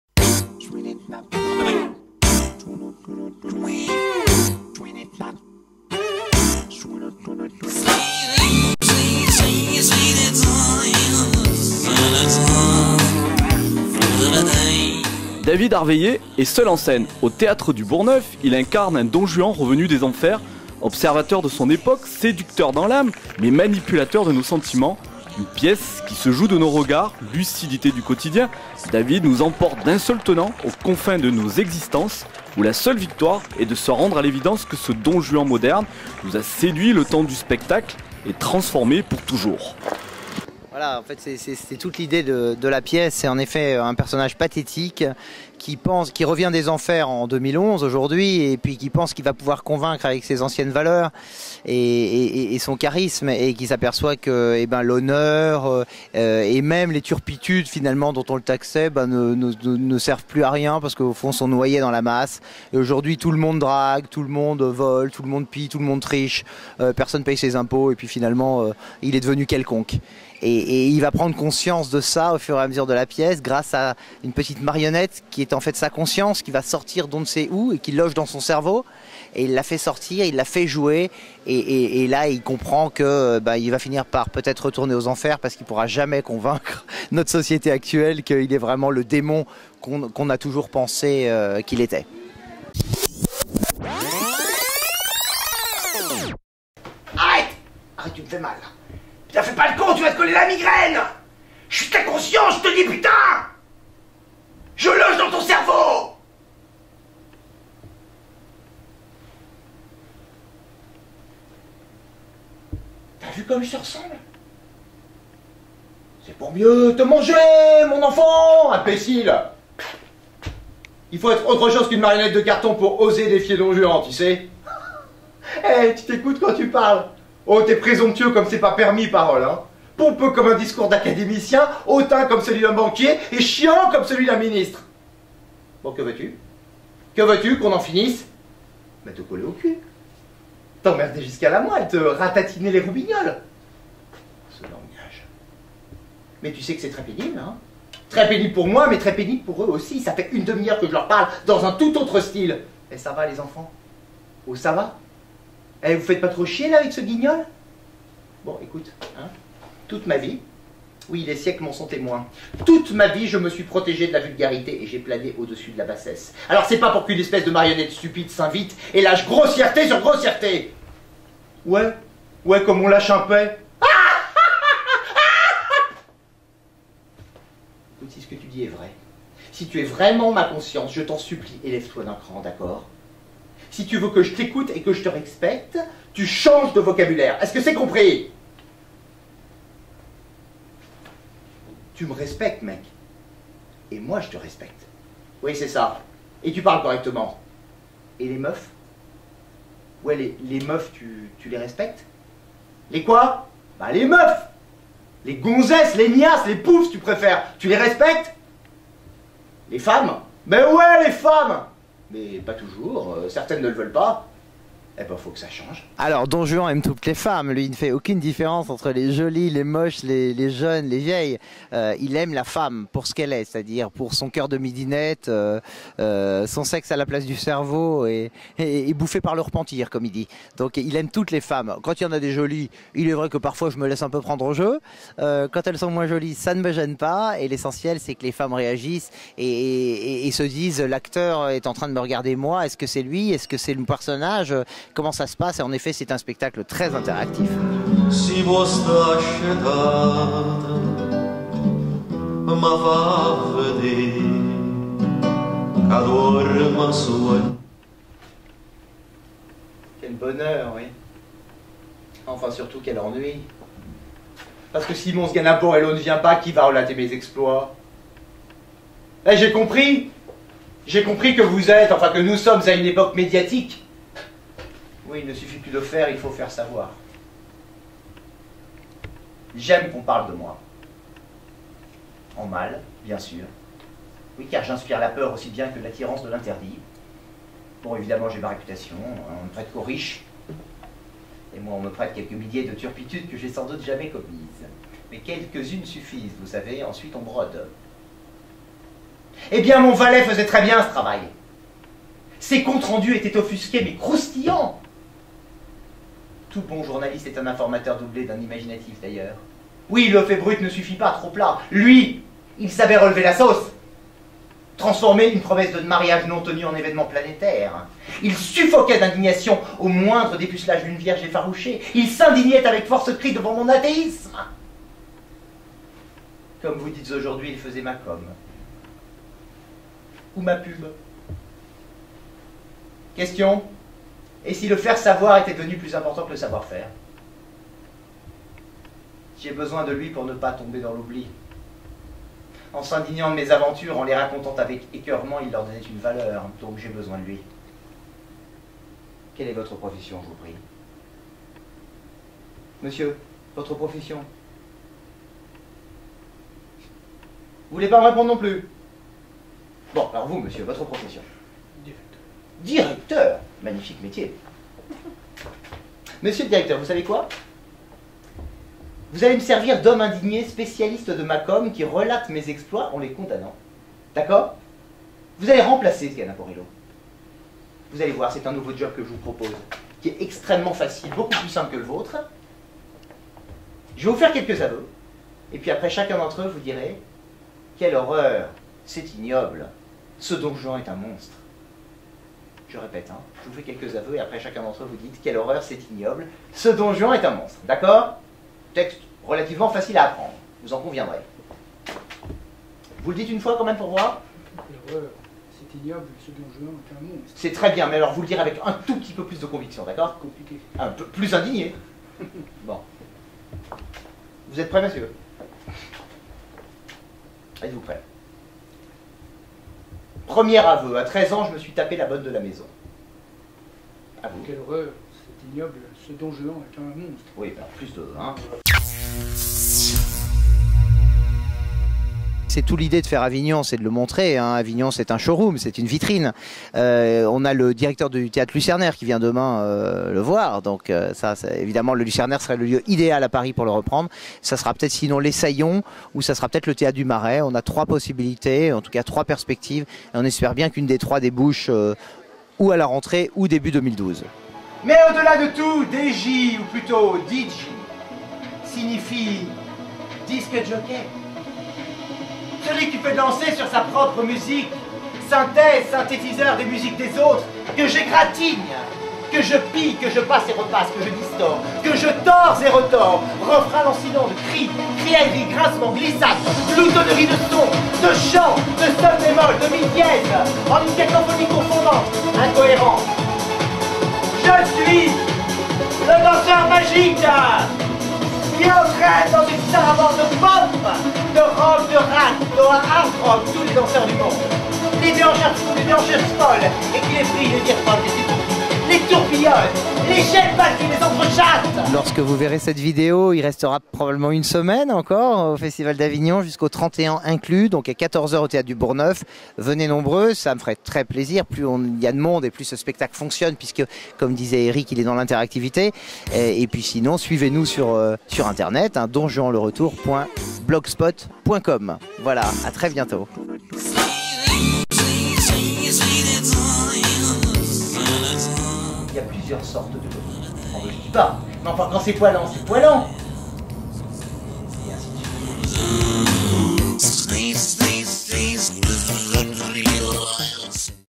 AVIGNON OFF 2011 & Don Juan le retour (David ARVEILLER)
Prouesse de scène pour cet acteur de théâtre, qui incarne un Don Juan fouineur de nos consciences, sortant des flammes de l'enfer. réactivant nos mémoires pour notre bien comme pour notre mal. Un spectacle qui nous laisse un regard sur nous-même et une force théâtrale incarnée magistralement par David ARVEILLER somptueux !!!